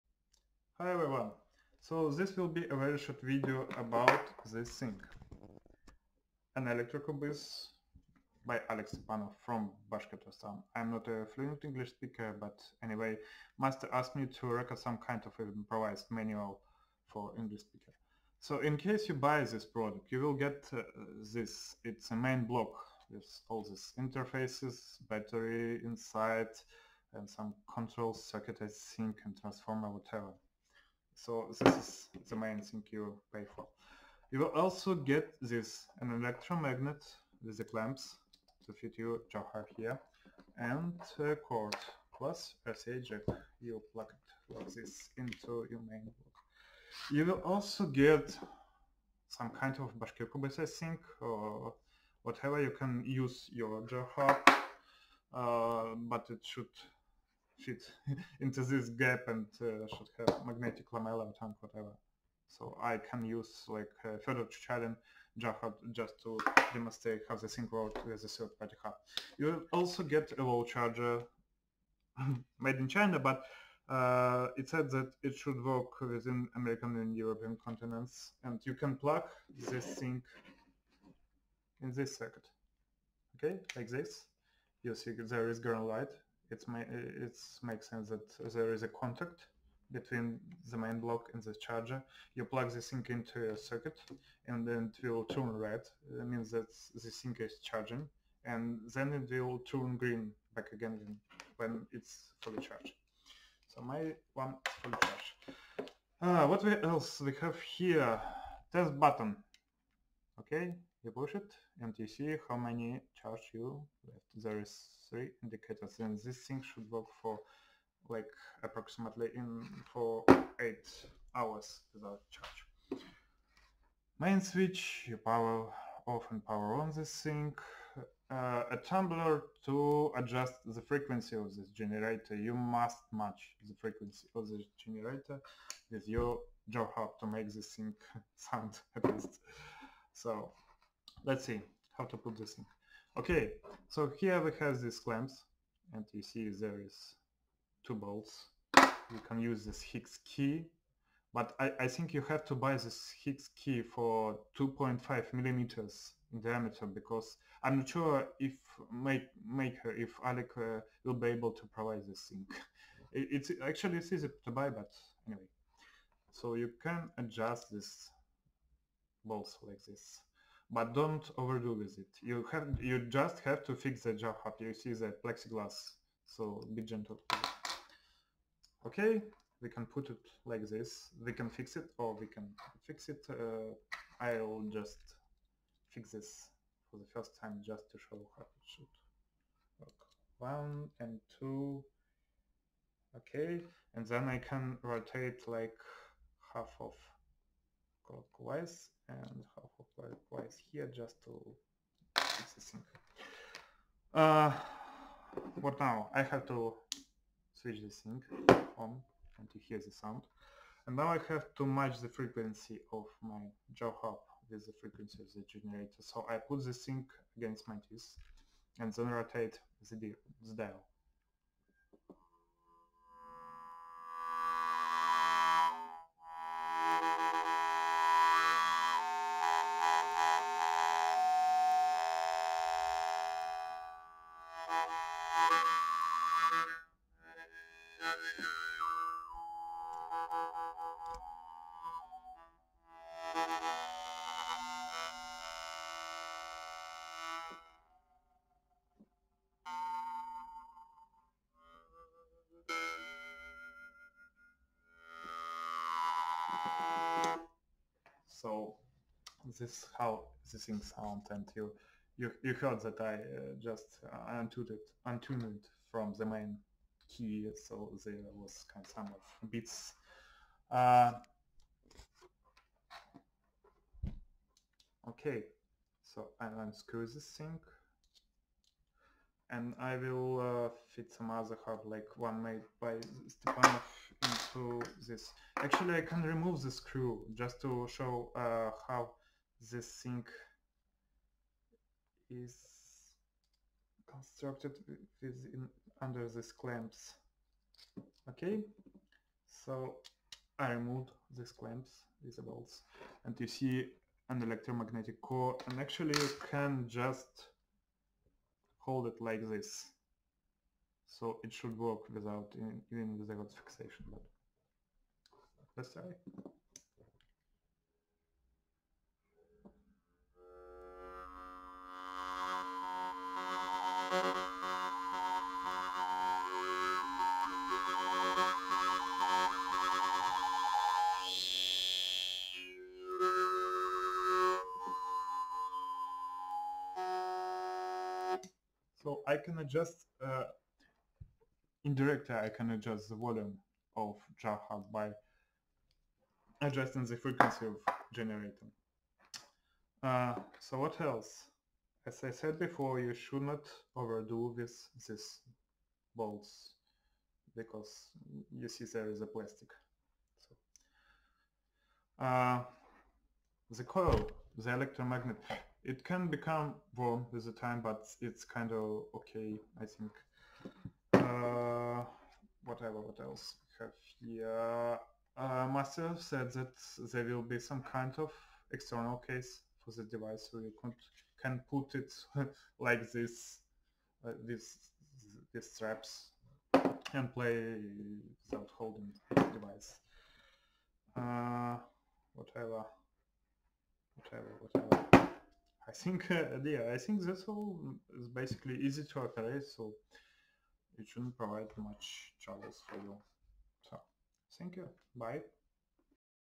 How-To: Electro-kubiz by Alik Stepanov (English)
A short improvised manual for an electro-kubiz by Alik Stepanov, webcam recording.